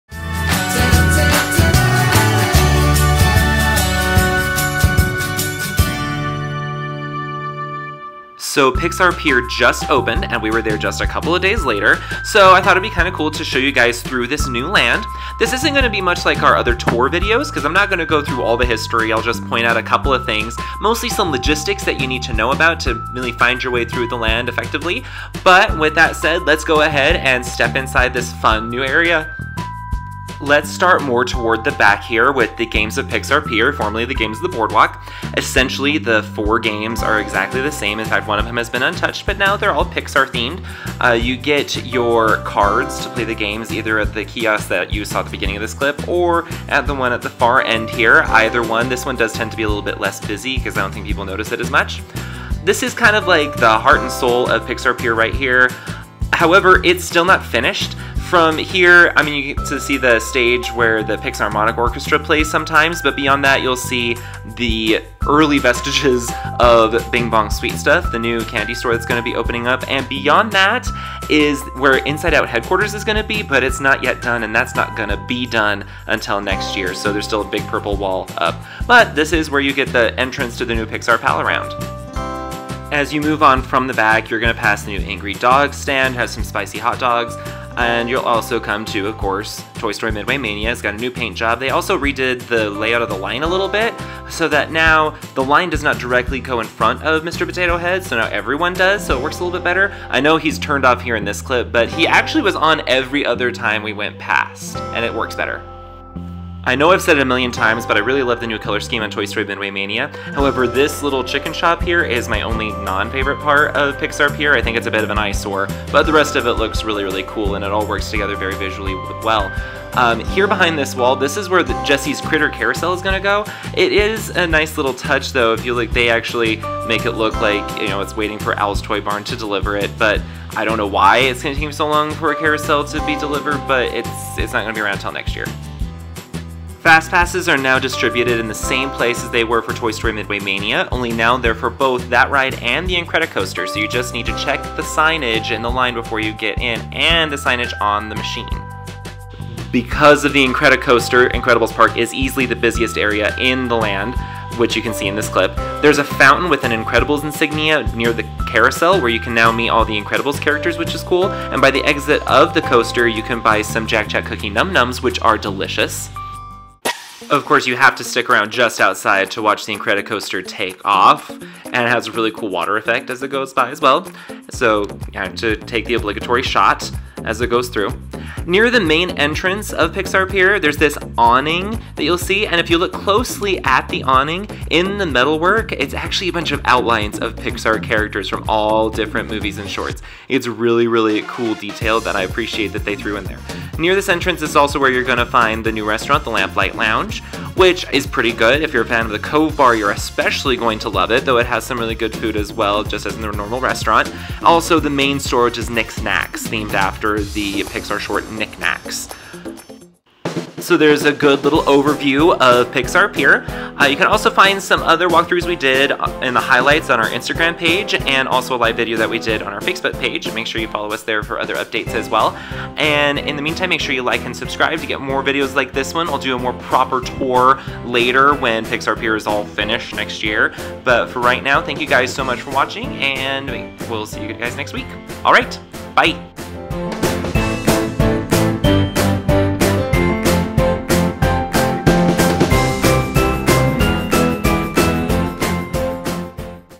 A Quick Introduction to Pixar Pier
Pixar Pier has now officially opened at the Disneyland Resort! Although it's not completely finished yet, here's a quick introduction and overview of the new pier so you can get your bearings, see all the fun in store, and figure out where everything is now!

for more tips, tricks, hacks, secrets, news and more!